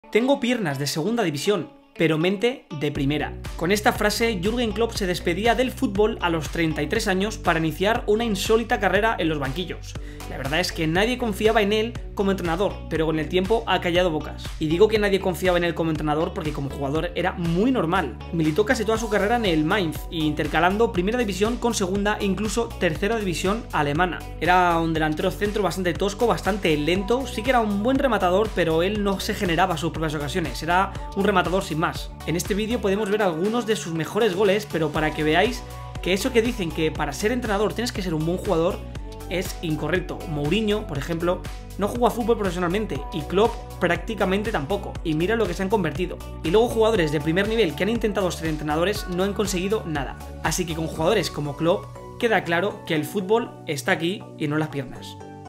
ASÍ JUGABA KLOPP 🤯⚽
Jürgen Klopp fue futbolista. La verdad es que era un jugador muy mediocre pero como entrenador lo ha petado.